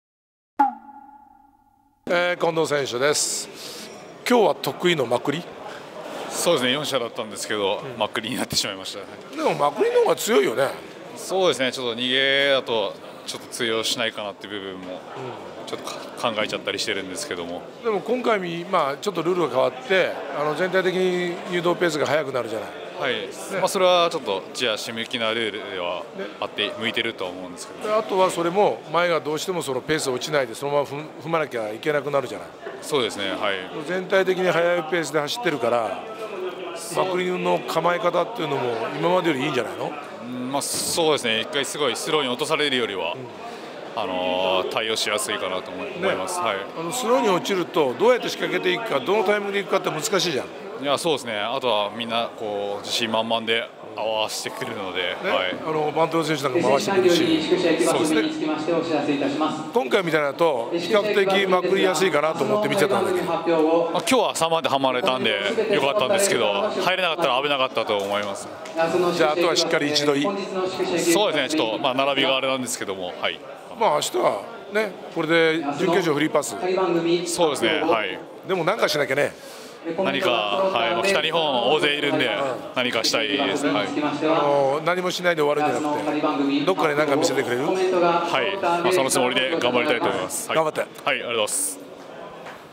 近藤 隆司【本気の競輪TV】中野浩一の【第70回高松宮記念杯競輪GⅠ】白虎賞・青龍賞注目選手インタビュー
【本気の競輪TV】中野浩一の【第70回高松宮記念杯競輪GⅠ】白虎賞・青龍賞注目選手インタビュー